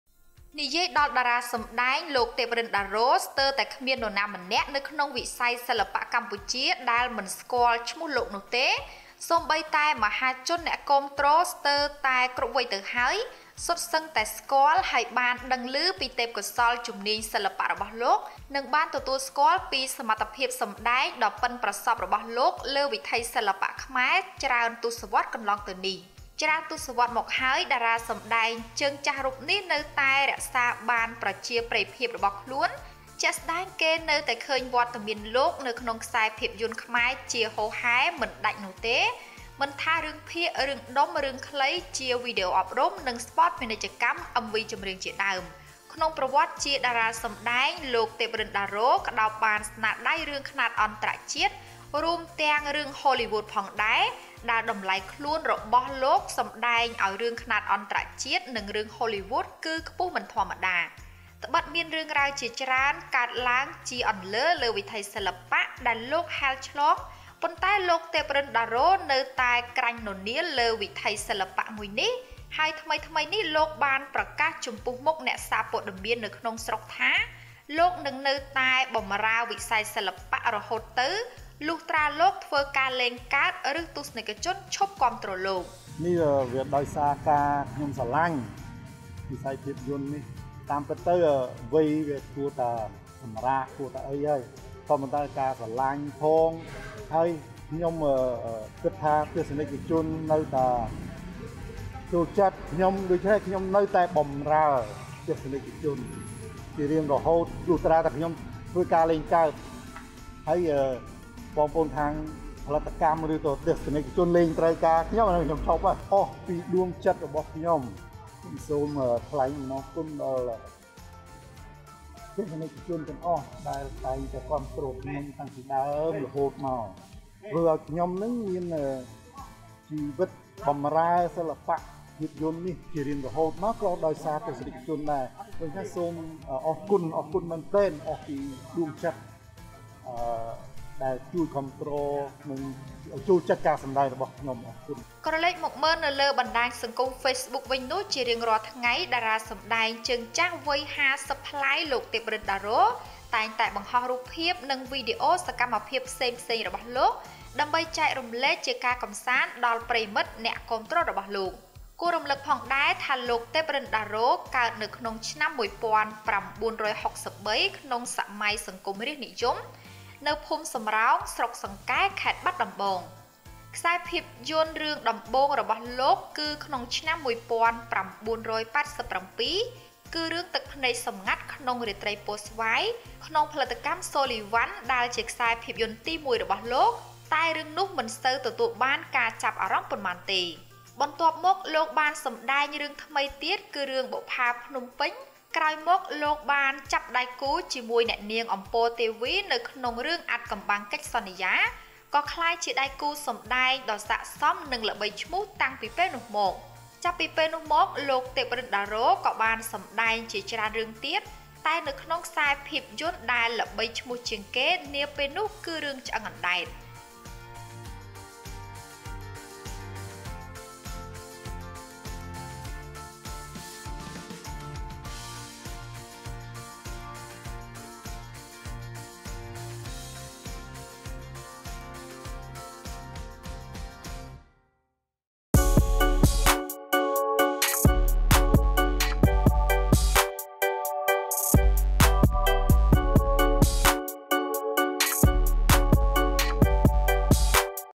[24-07-21] ទេព រិនដារ៉ូ ប្រកាសបម្រើវិស័យសិល្បៈរហូតលែងមានអ្នកគាំទ្រទើបឈប់
ទេព រិនដារ៉ូ ប្រកាសបម្រើវិស័យសិល្បៈរហូតលែងមានអ្នកគាំទ្រទើបឈប់